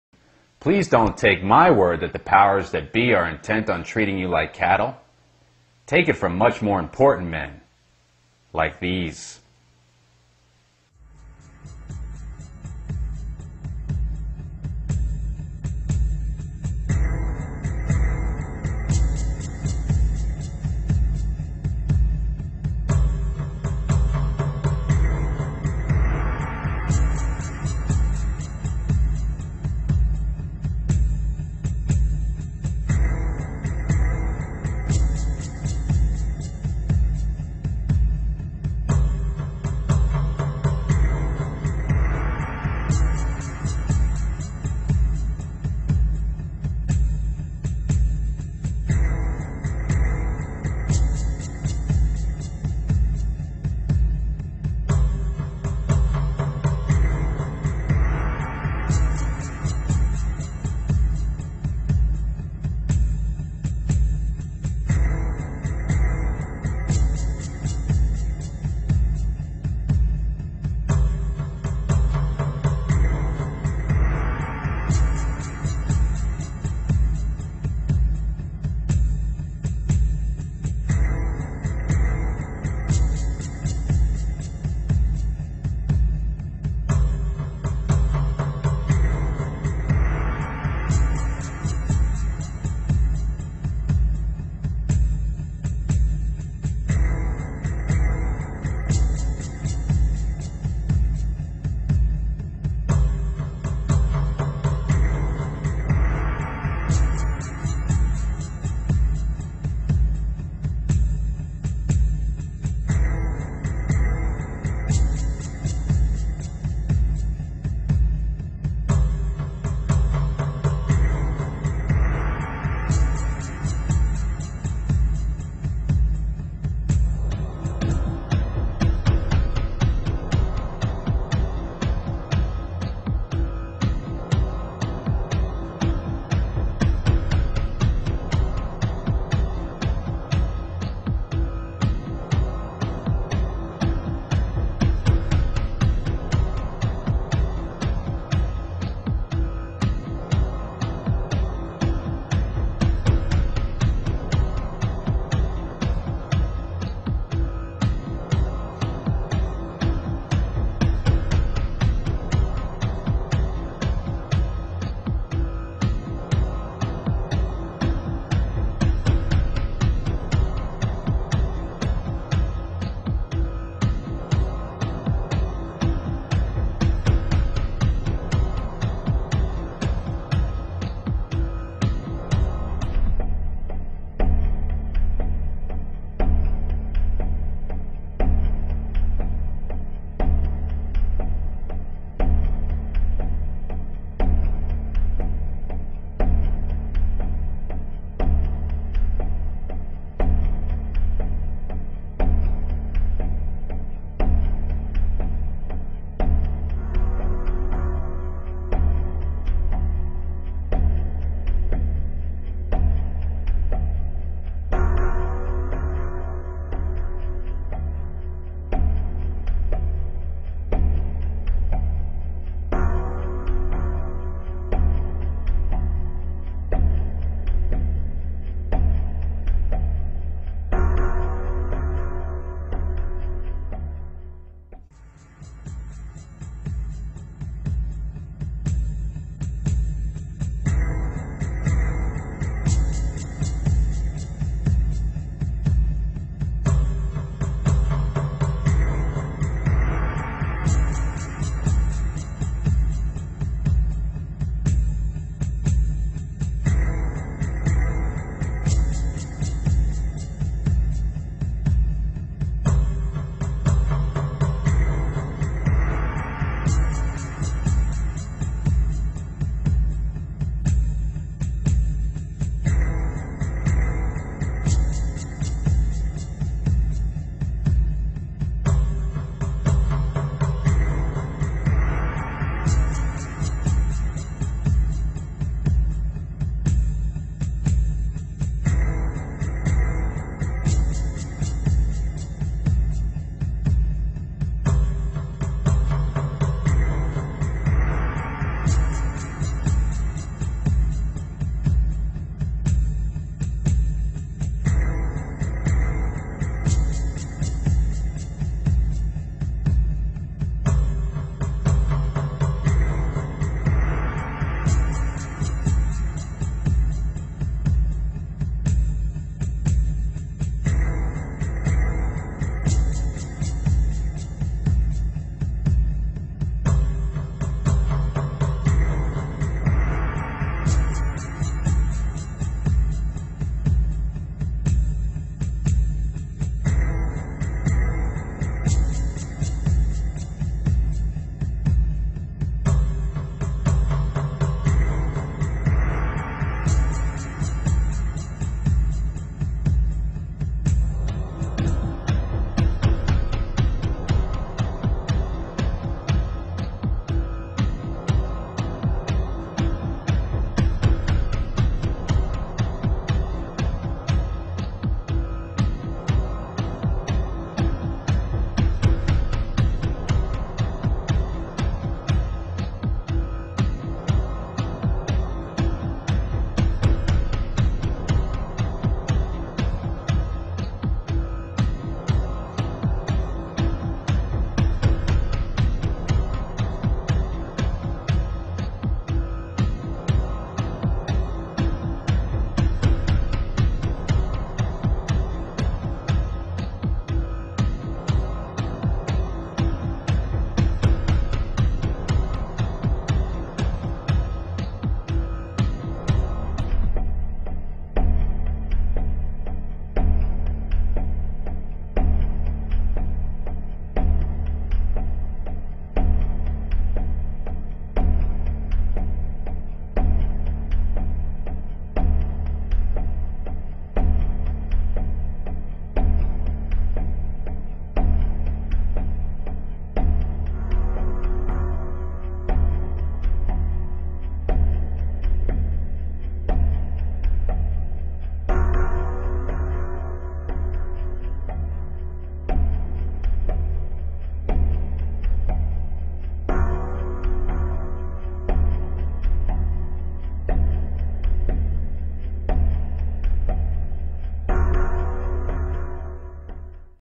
World Leaders ADMIT to Conspiracy - New World Order - NWO Totalitarian Control
World Leaders ADMIT there is a Conspiracy. QUOTES from politicians, Presidents and other world leaders about those who really have all of the power and control over all of us, including the world leaders themselves! The real powers do not want to be known to the public and stay hidden, but don't believe ME, believe much more important people, like these...